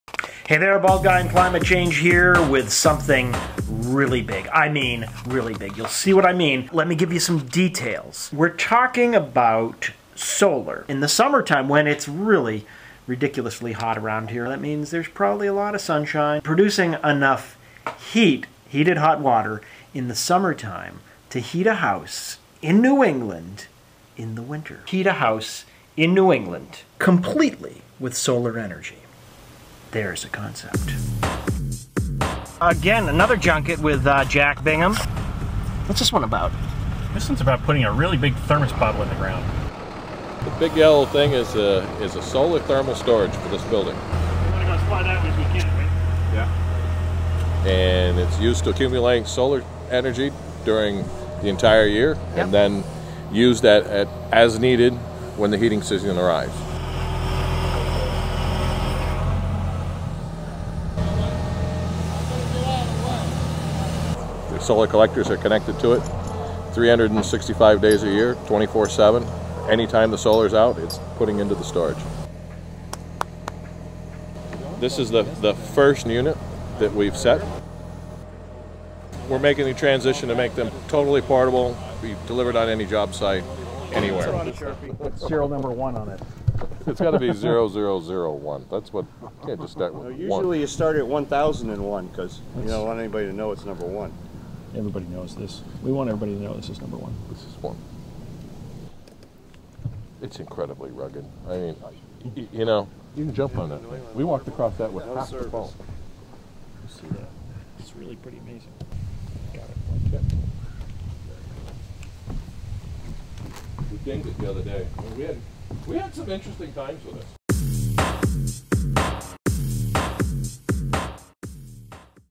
Bald Guy 047.mov
The next big thing in energy actually is pretty big. And yellow. Jack Bingham of the SEA Solar Store takes Bald Guy's Bill Rogers to see the first in the world install of a solar thermal tank capable of heating a house in New England -- year round -- with solar energy. That's handy because it tends to get cold in the New England winter when there's not much sunlight. Vermont's Thermal Storage Solutions puts the heat in a huge, hugely insulated tank of water and gives the heat to the house when it's needed. Store it in the summer. Give it up in the winter. Pretty cool. Or hot.